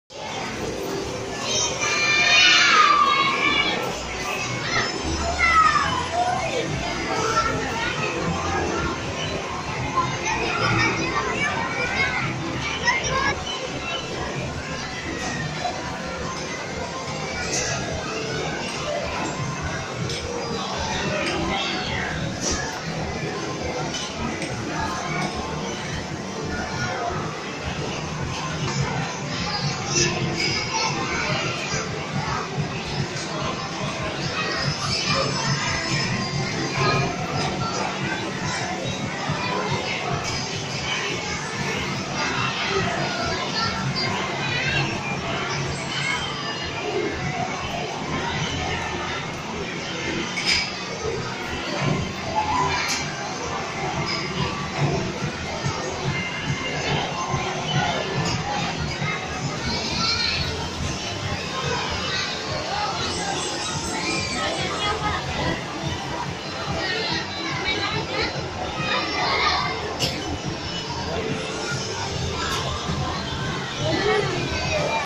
TUKAR TIKET DENGAN HADIAH
Ini kumpulan tiket yang dikumpulkan sejak beberapa bulan yang lalu dan hasilnya sekitar 1100 tiket yang dapat ditukar dengan beberapa pilihan hadiah di FunCity Mesra Indah Mall Samarinda.